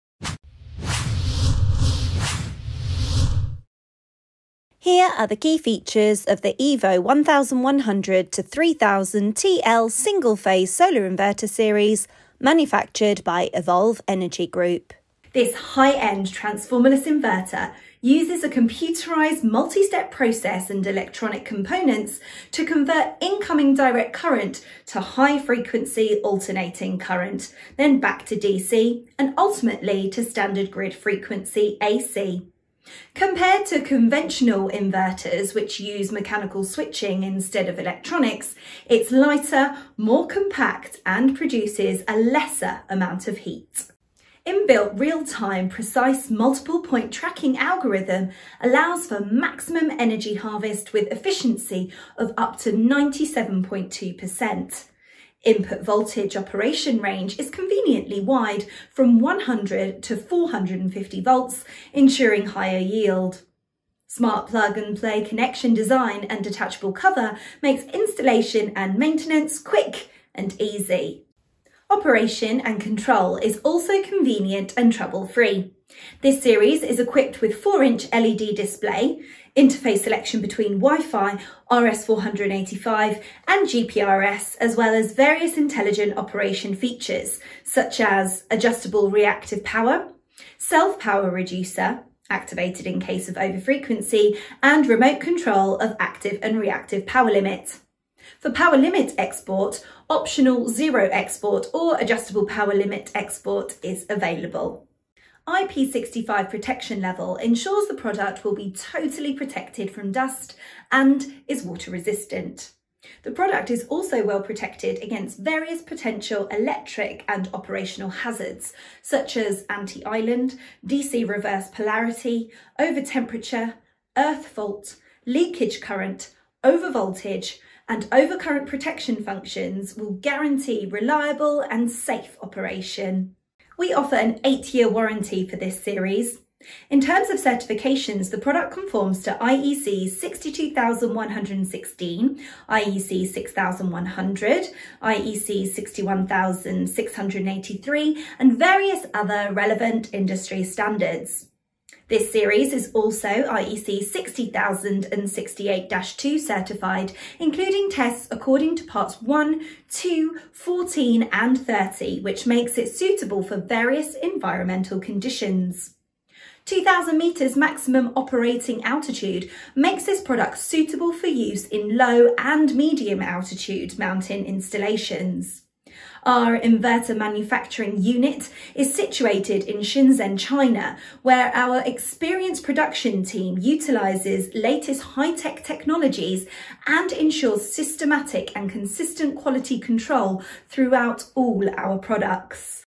Evolve Energy Group - EVVO 1100-3000 TL Solar Inverter Series
𝐃𝐞𝐬𝐜𝐫𝐢𝐩𝐭𝐢𝐨𝐧:
● Detachable Cover for Easy Installation
● Rust-free aluminum covers
● Flexible monitoring solution
● Max97.2% efficiency
● Real time precise MPPT algorithm for max harvest
● Wide input voltage operation range

𝐊𝐞𝐲 𝐋𝐢𝐧𝐤𝐬:

Product profile for Evolve Energy EVVO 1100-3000 TL Inverter including product datasheet